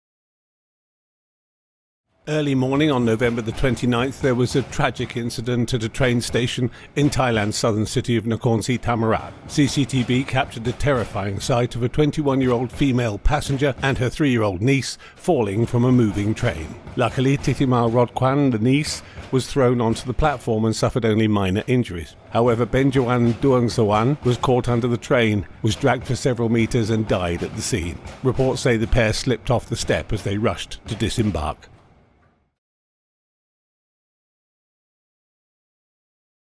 TRAIN TRAGEDY | Ch3Thailand
Early morning on November the twenty ninth, there was a tragic incident at a train station in Thailand’s southern city of Nakhon Sithammarat. CCTV captured the terrifying sight of a twenty one year old female passenger and her three year old niece falling from a moving train. Luckily, Thitima Rodkhwan, the niece, was thrown onto the platform and suffered only minor injuries. However, Benjawan Duangsuwan was caught under the train, was dragged for several meters and died at the scene. Reports say the pair slipped off the step as they rushed to disembark.